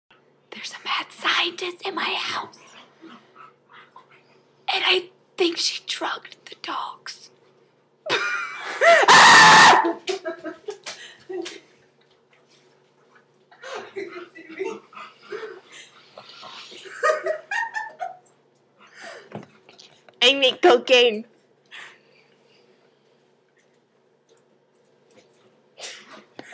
Free cocaine in this house; coke all around!
A mad scientist invades our house, makes cocaine, drugs the dogs. all while looking sexy in a lab coat & goggles.